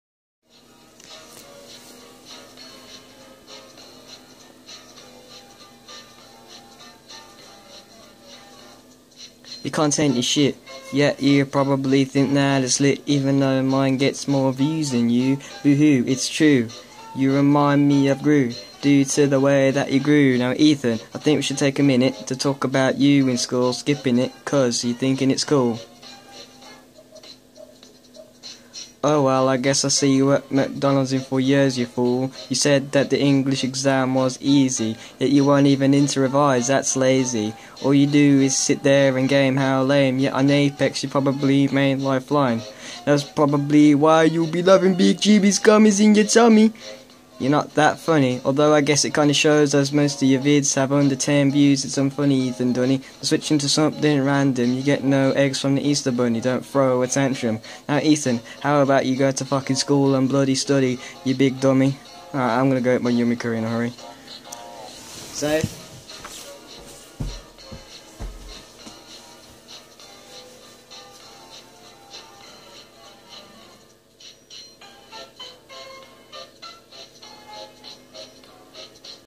Diss track on youknowwho
He wanted me to make a Diss track for the jokes, so here it is. It's probably shit but I don't do this kind of thing so oh well.